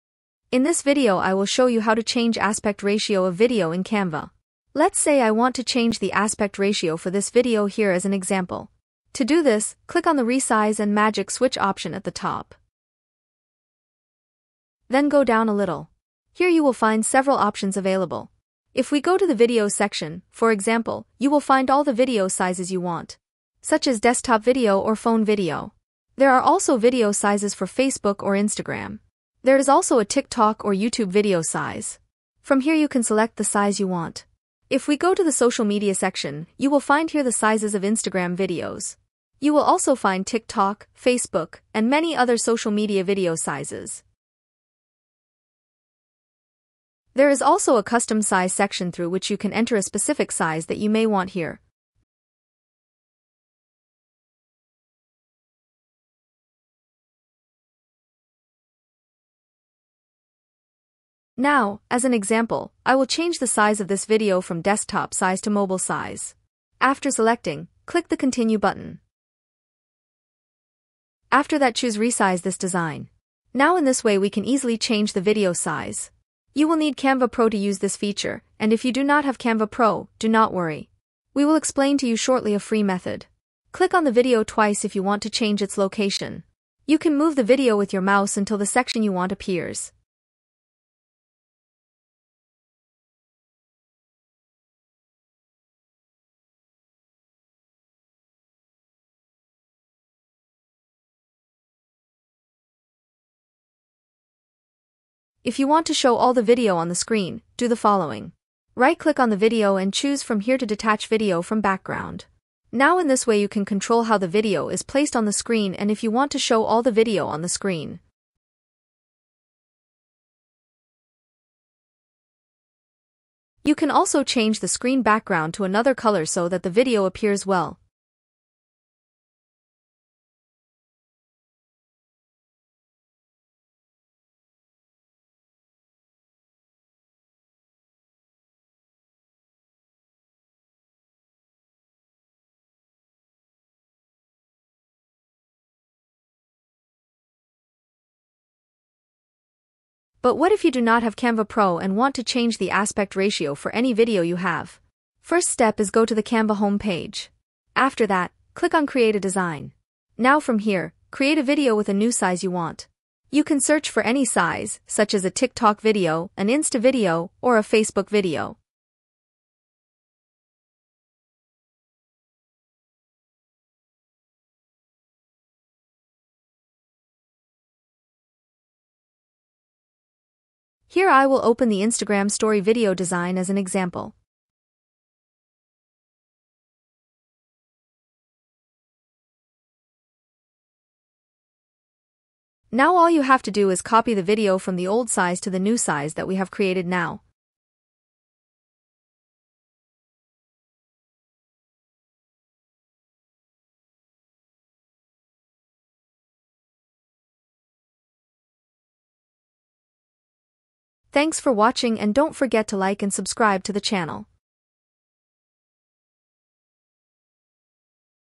How To Change Aspect Ratio of Video in Canva
In this comprehensive tutorial, I'll show you the exact steps to effortlessly change the aspect ratio of your videos using Canva's intuitive platform.